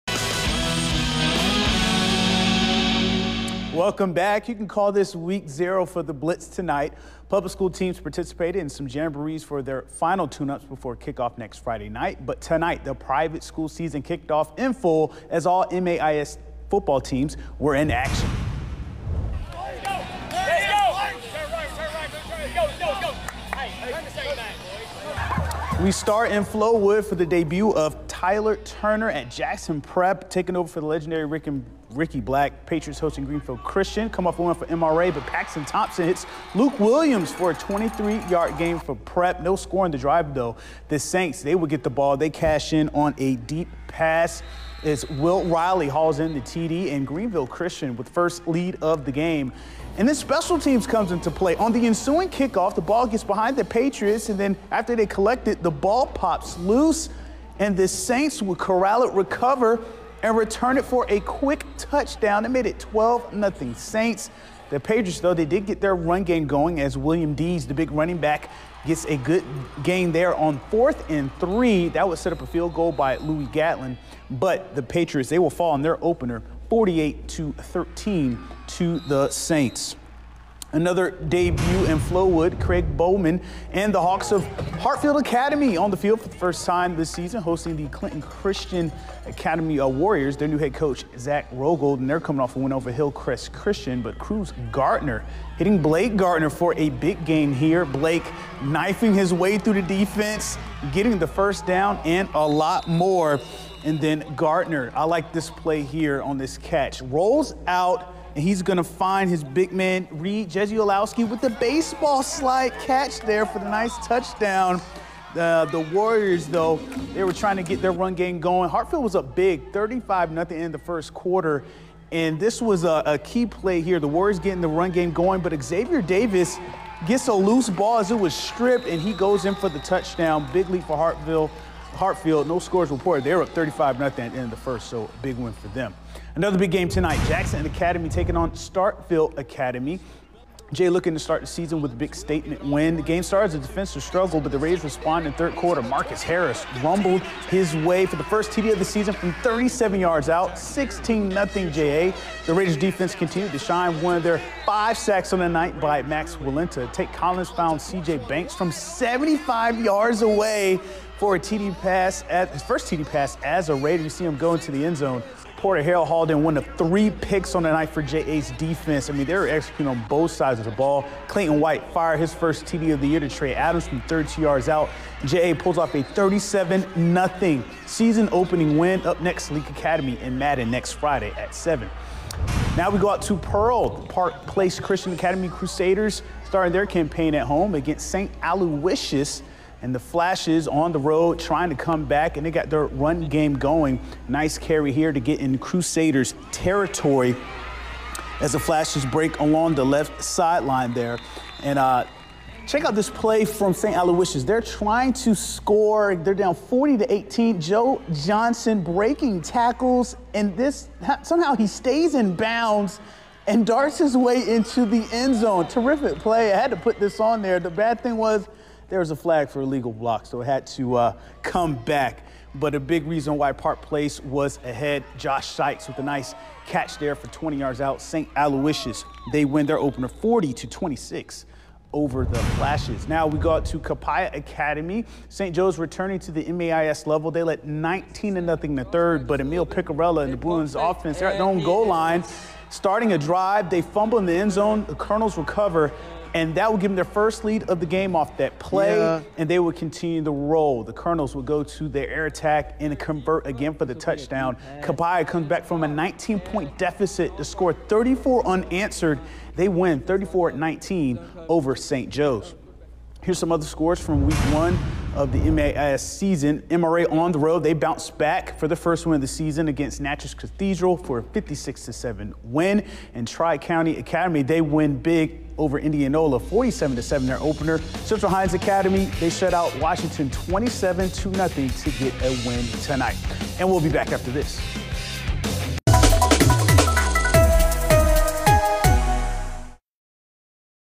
Mini Blitz: MAIS Week 1 Recap
All MAIS teams took the field for Week 1 of their high school football season. Check out the highlights and scores from Friday night's action! Finals in this segment  Greenville Christian 43 Jackson Prep 13 CCA 7 Hartfield Academy 49 Starkville Academy 0 Jackson Academy 37 St. Aloysius 26 Park Place 40 MRA 56 Natchez Cathedral 7 St. Joe's 19 Copiah Academy 34 Central Hinds 27 Washington 0